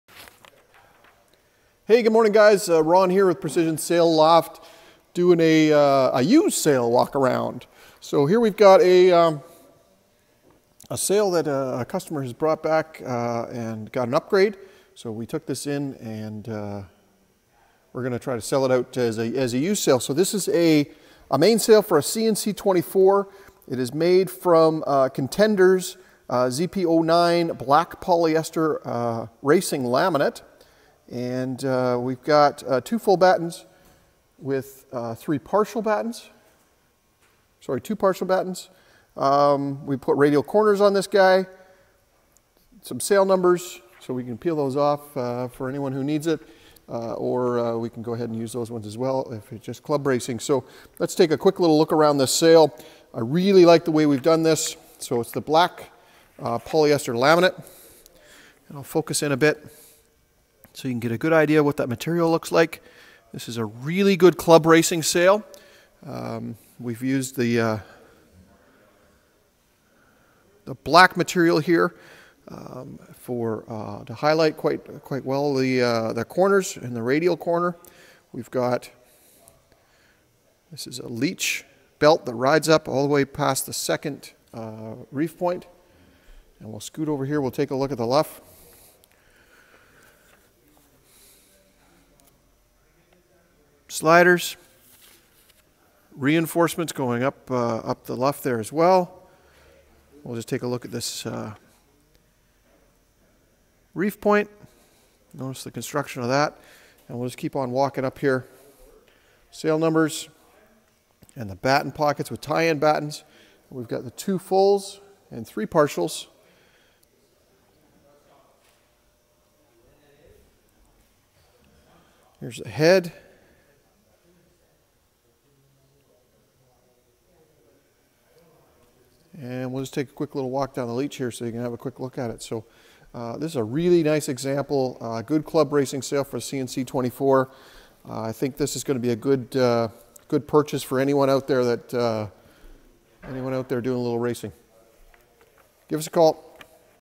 C&C 24 Mainsail - Used
C&C 24 Mainsail designed by Precision Sails and constructed from Contender Sailcloth's  ZP09 Polyester Racing Laminate.  Features include: 2 Full and 3 Partial Battens, 1 Reef Point, Offshore Package including Luff, Leach and Reef Belts, Radial Reinforced Corners, C&C Insignia and Sail Numbers